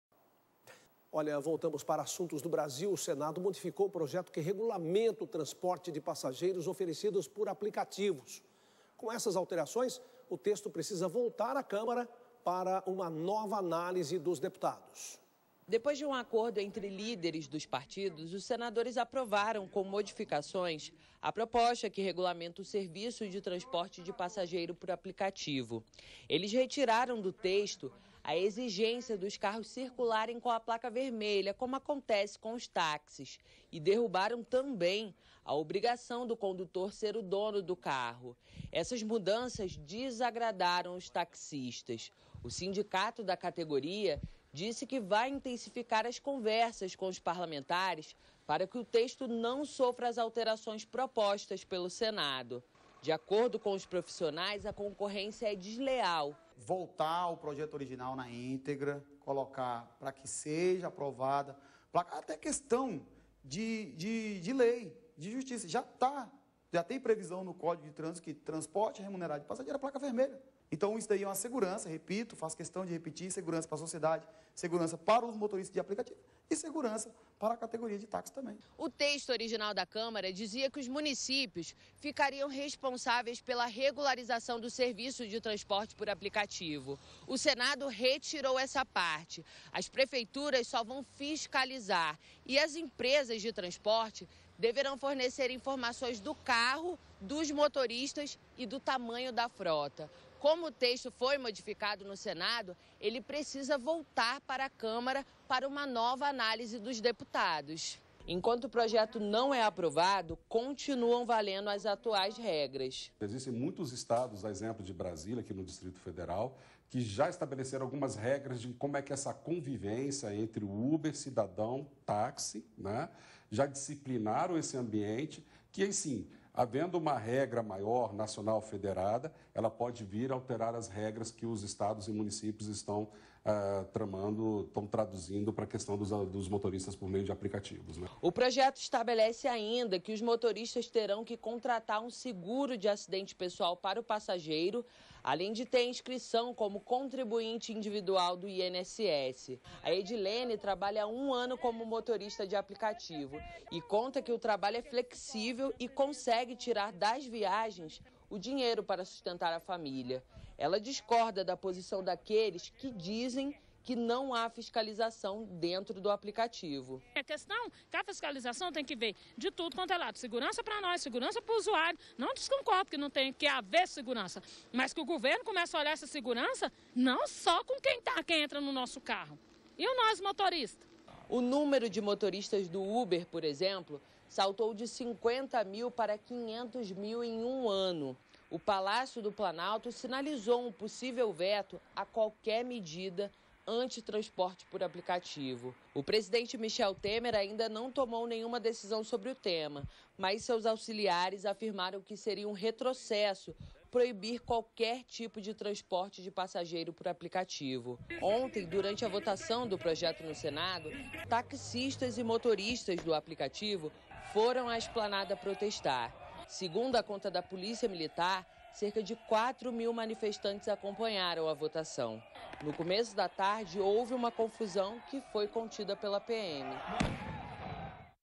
Senado modifica projeto que regulamenta transporte oferecidos por aplicativos
O Senado modificou o projeto que regulamenta o transporte de passageiros oferecidos por aplicativos. Com essas alterações, o texto precisa voltar para a Câmara para uma nova análise dos deputados